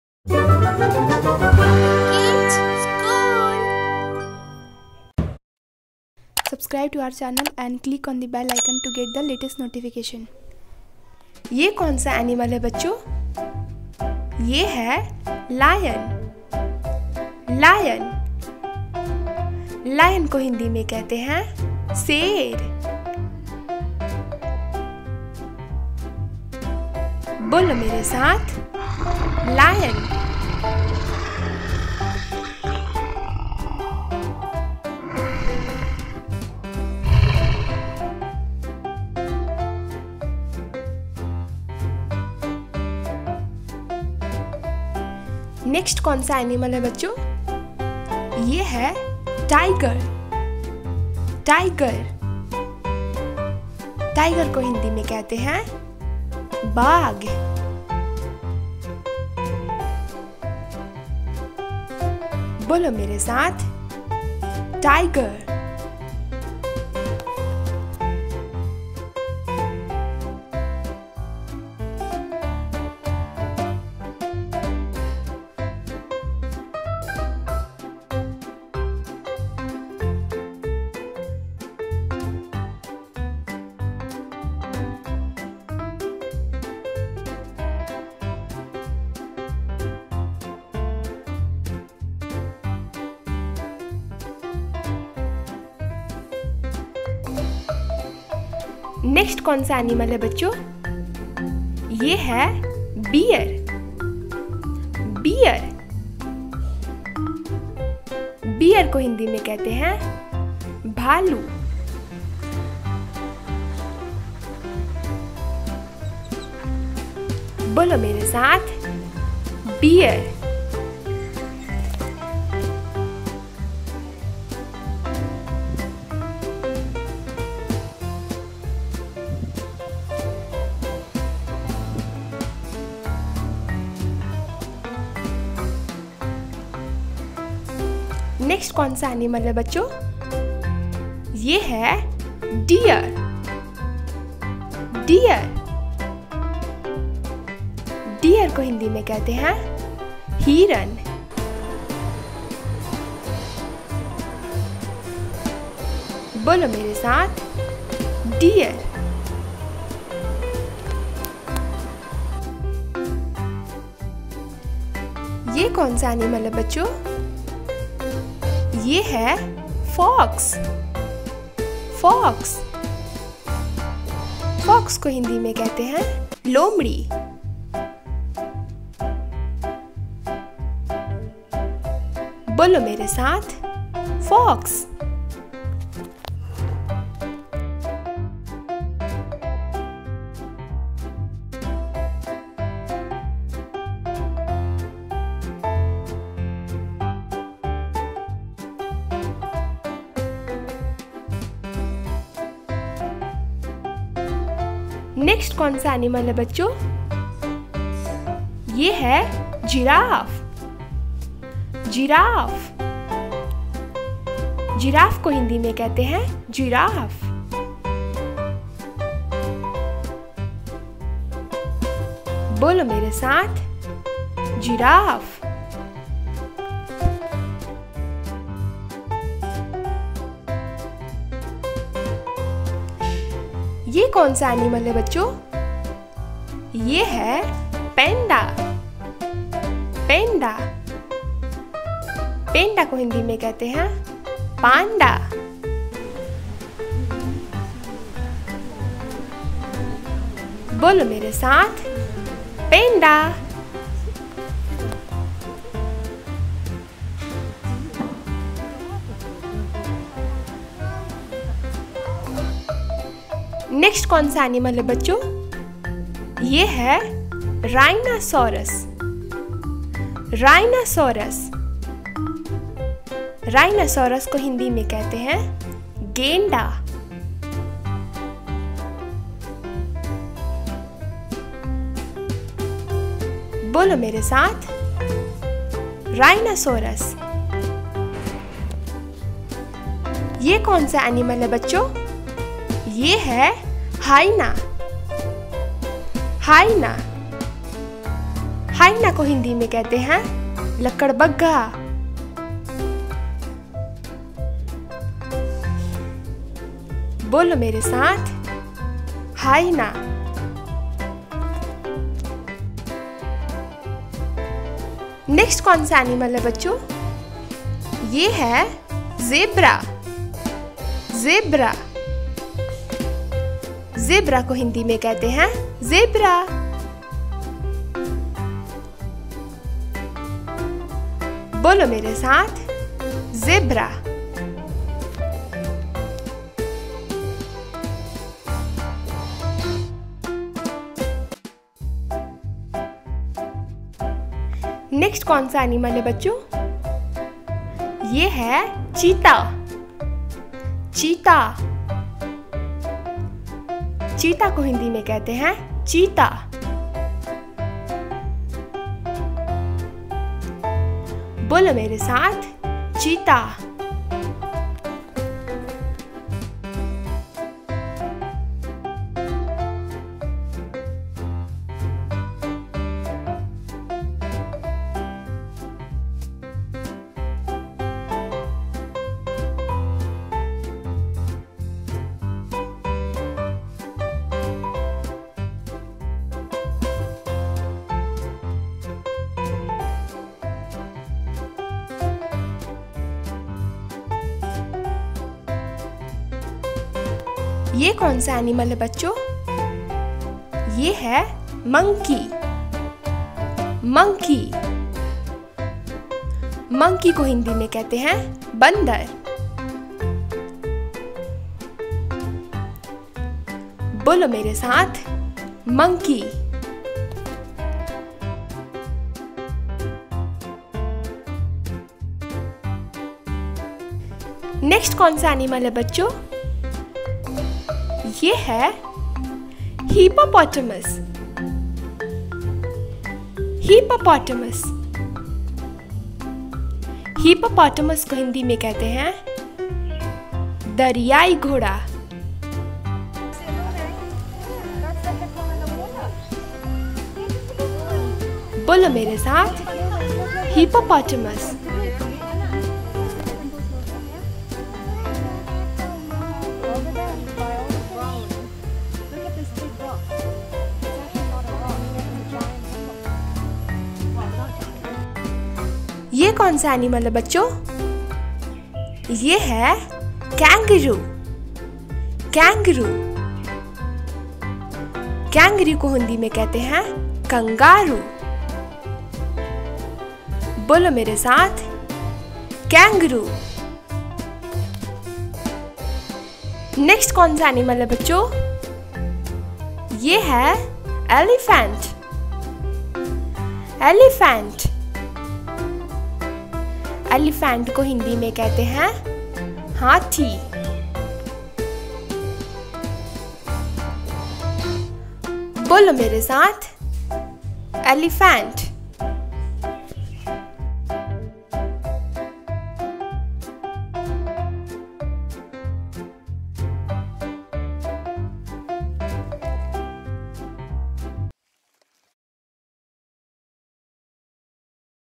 Learn Wild Animals Name Hindi & English With Live Examples | जंगली जानवरो के नाम
Hello Viewers,
This video will help you to learn Wild Animals name in Hindi & English with live examples. We have added video clips of Animals that makes the video more intresting and a must watch.
From this video you will learn
1. Names of Wild animals
2. Wild life animals
3. Wild Animals Hindi and english with video clips
Enjoy the video, have a happy time learning.

Our Chapters:
0:00-0:10  Introduction
0:11 - Lion
0:45 - Tiger
1:38 - Bear
2:18 - Deer
2:45 - Fox
3:24 - Giraffe
3:53 -Panda
4:23 -Rhinoceros
4:50 -Hyena
5:15 -Zebra
5:45 -Cheetah
6:37 - Monkey
7:04 -Hippopotamus
7:45 -Kangaroo
8:05 -Elephant